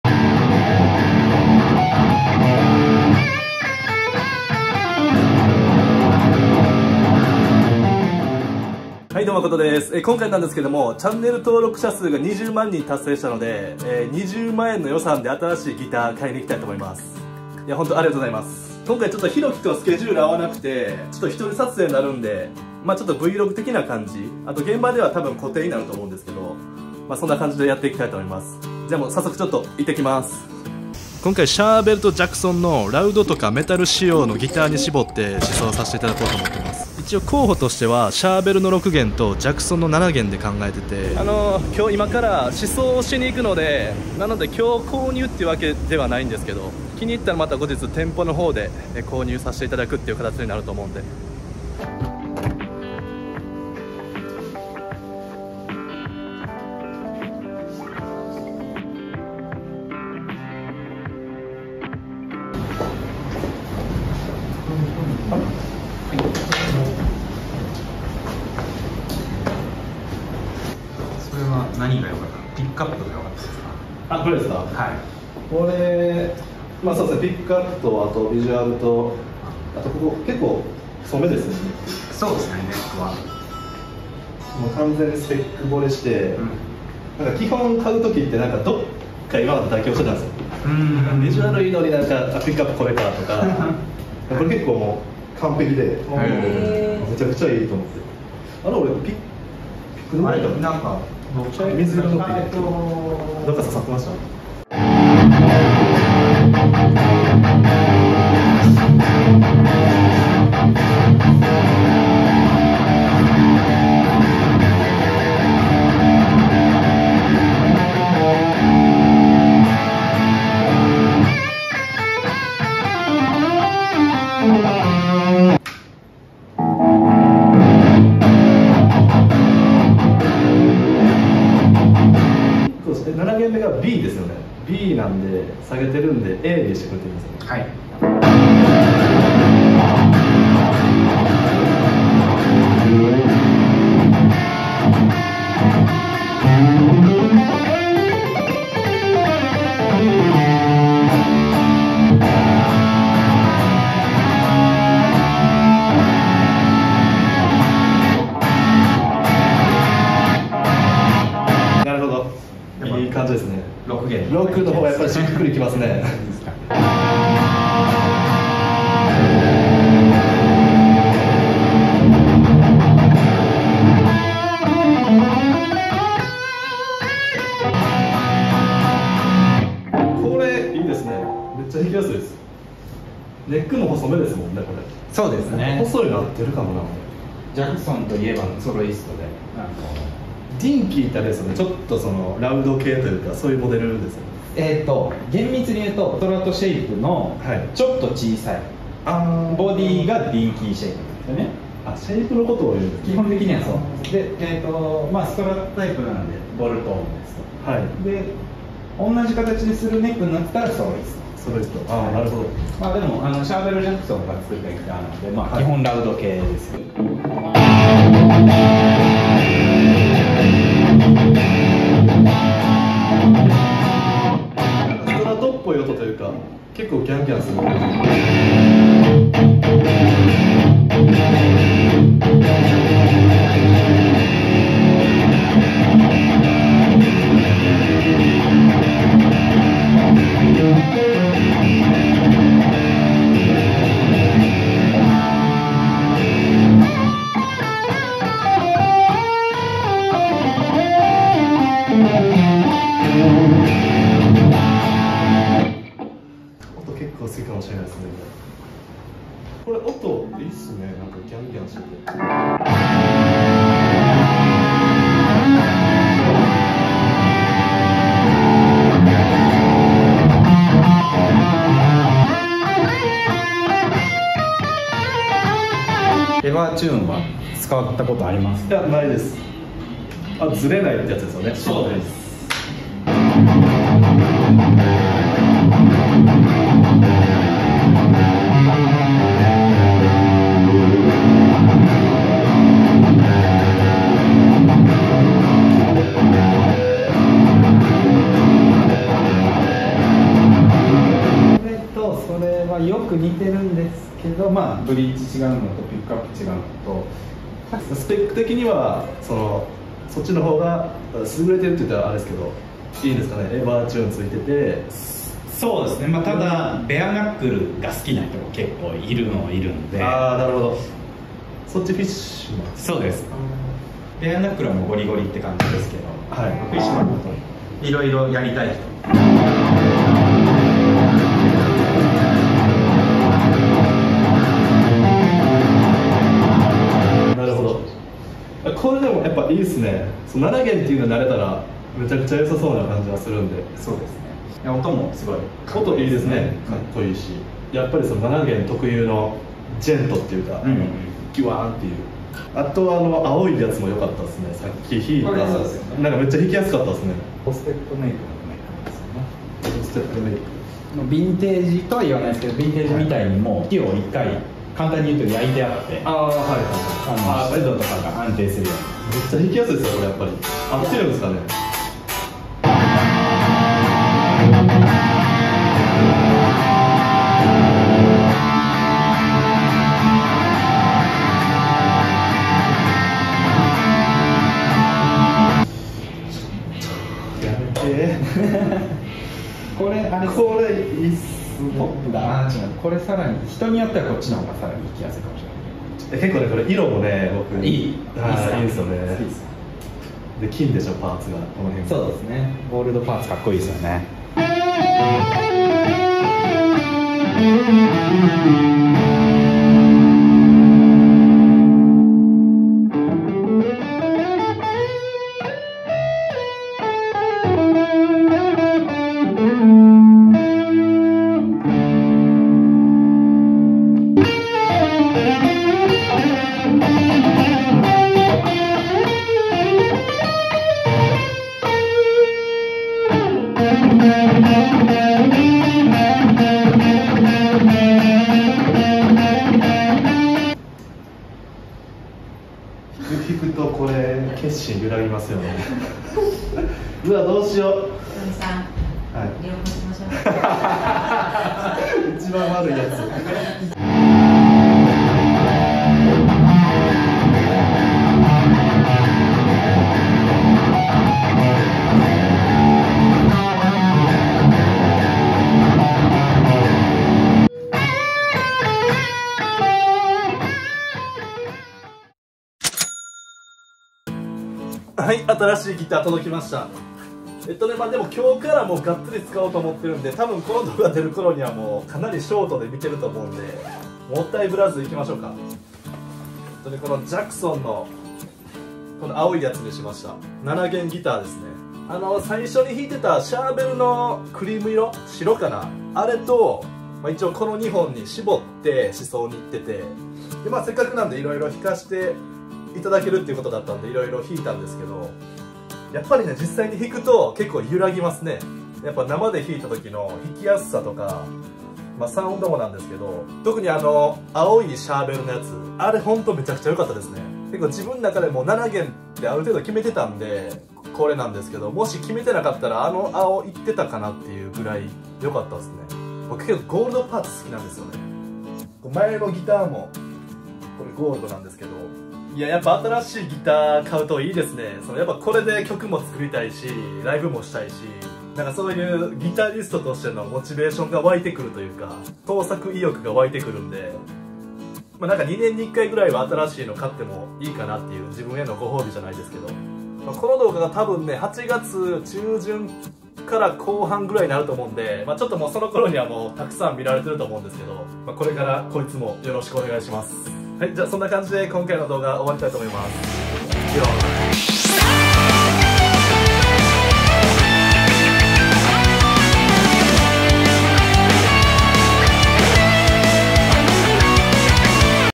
ゴリゴリなメタル向けギターを買いに行く【Charvel / Jackson】
オリジナル曲のプレイリスト

Makoto Fukamiのグッズはこちら。

0:00 オープニング
0:52 到着！
1:44 Charvel Pro-Mod DK24 HH HT E Mahogany with Poplar Burl
2:05 Jackson Pro Plus Series DK Modern MDK7P HT
2:26 Jackson Pro Series Dinky® DK Modern HT6 MS
2:46 Jackson Pro Plus Series Soloist™ SLA3
3:07 SoloistとDinkyについて
3:36 Charvel Pro-Mod DK24 HSH 2PT CM
4:36 Jackson Pro Plus Series DK Modern EVTN7
5:01 7弦ギター比べ
5:52 Charvelのギターが気になる…
7:20 決心が揺らぐ
7:54 新しいギターGET！